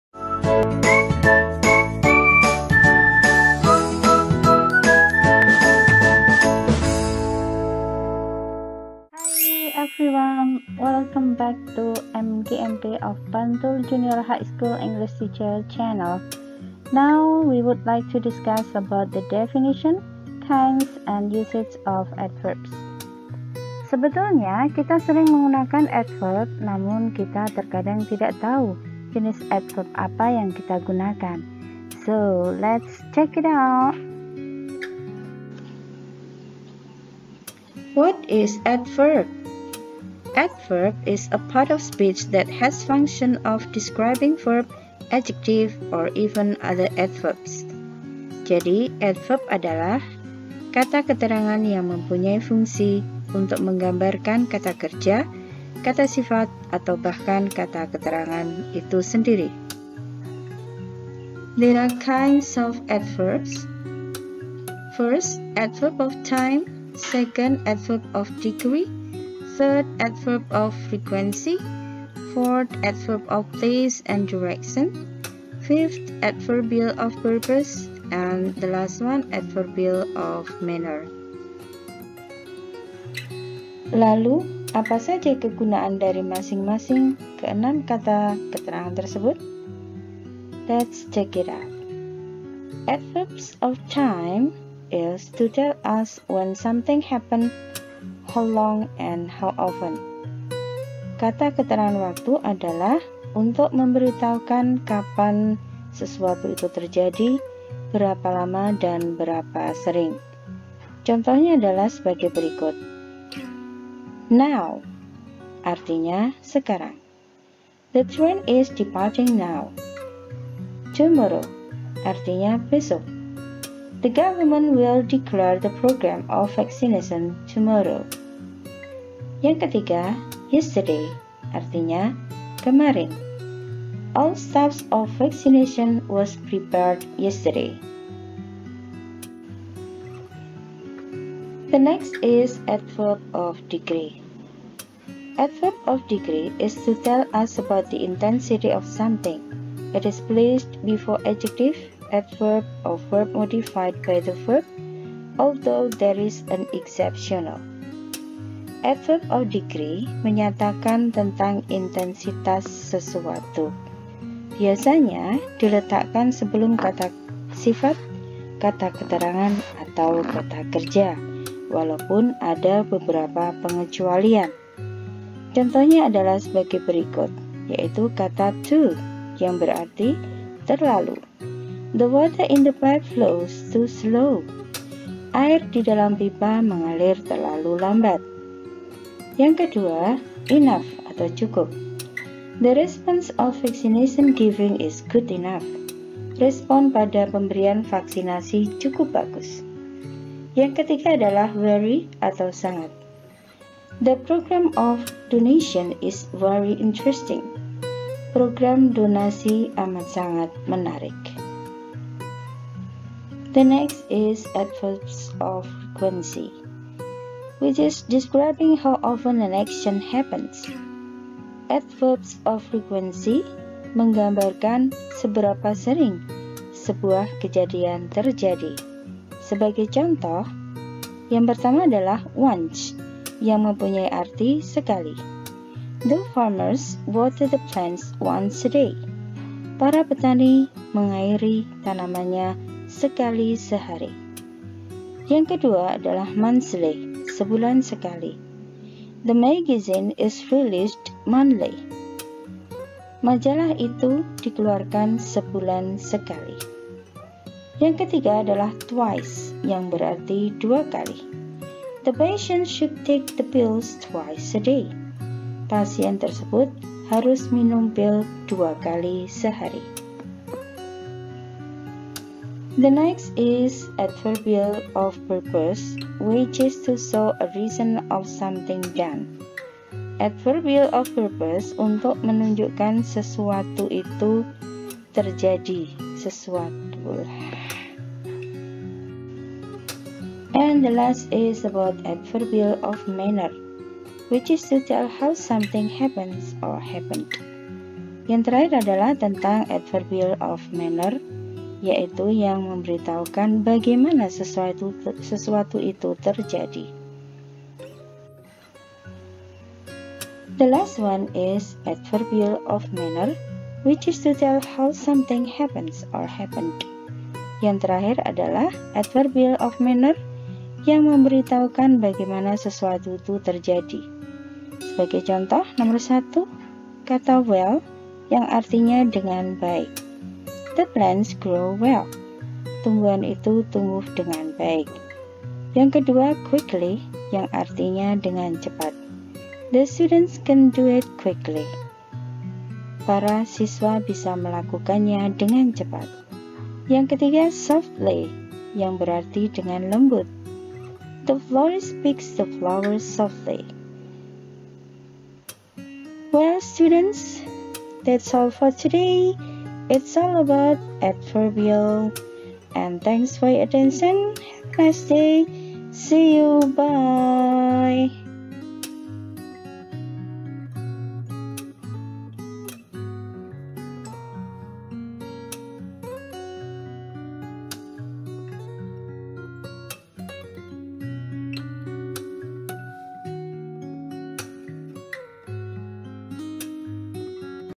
6 JENIS ADVERBS "Kata Keterangan" dan CONTOH PENGGUNAAN
Video ini berisi jenis jenis Adverbs (Kata Keterangan) dan cara menggunaknnya